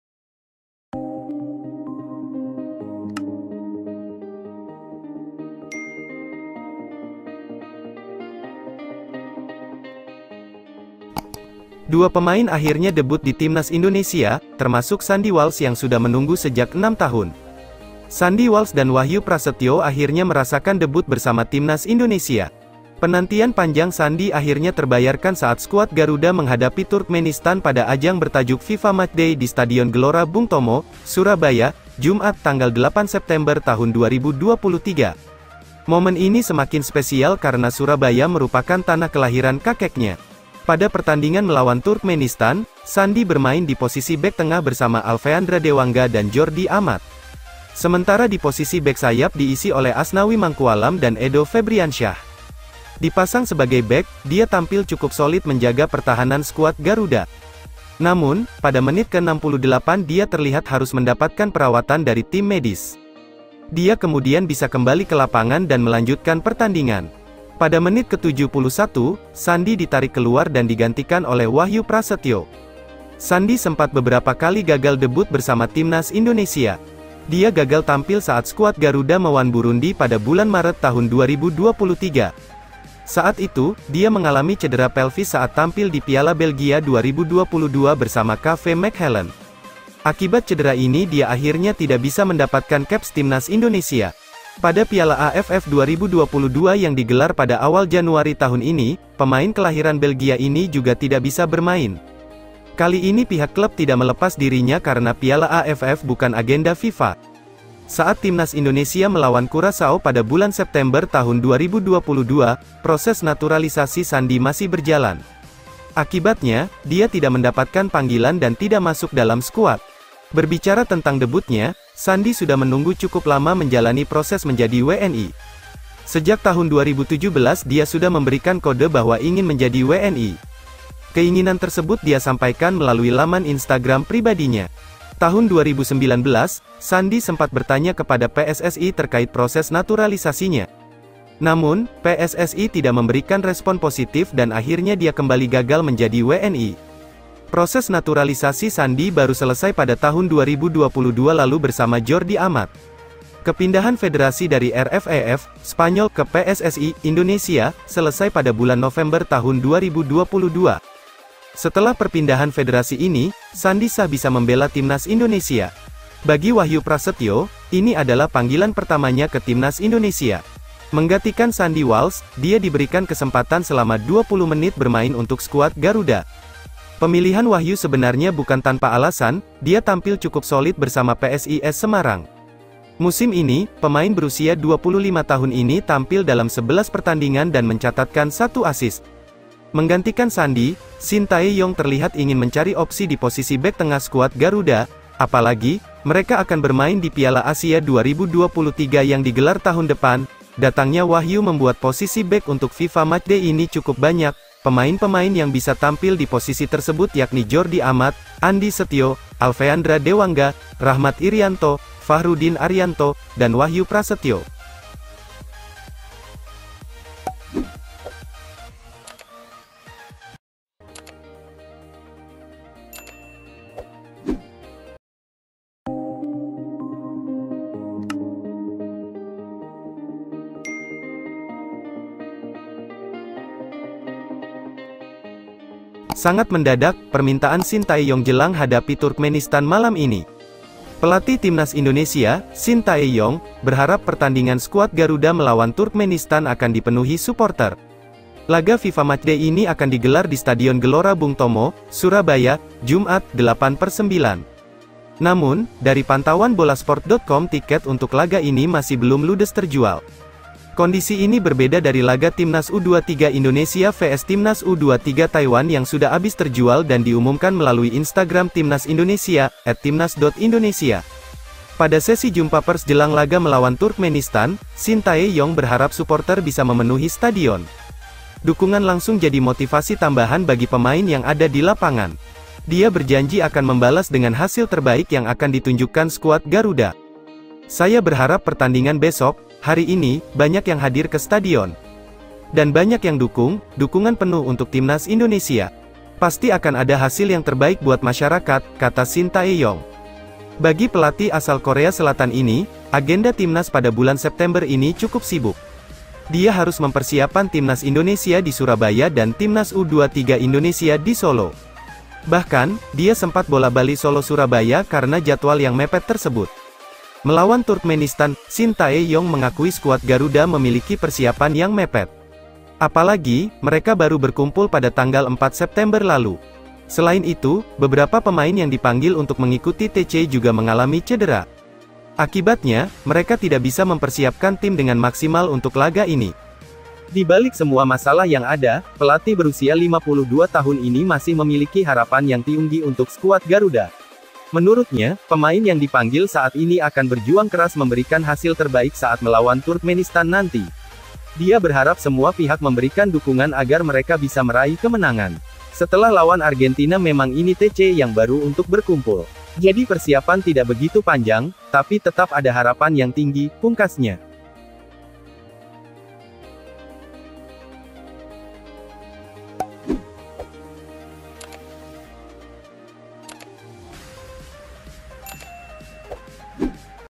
JADI PERBINCANGAN DI EROPA 🔴DEBUT INDAH SANDI WALSH BUKTI KAN KELASNYA, TIMNAS MELEJIT NAIK LEVEL✅
deskripsi:
Dua Pemain Akhirnya Debut di Timnas Indonesia, Termasuk Sandy Walsh yang Sudah Menunggu Sejak 6 Tahun.

Sandy Walsh dan Wahyu Prasetyo akhirnya merasakan debut bersama timnas Indonesia.

Penantian panjang Sandy akhirnya terbayarkan saat skuad Garuda menghadapi Turkmenistan pada ajang bertajuk FIFA Matchday di Stadion Gelora Bung Tomo, Surabaya, Jumat (8/9/2023).

Momen ini semakin spesial karena Surabaya merupakan tanah kelahiran kakeknya.

Pada pertandingan melawan Turkmenistan, Sandy bermain di posisi bek tengah bersama Alfeandra Dewangga dan Jordi Amat.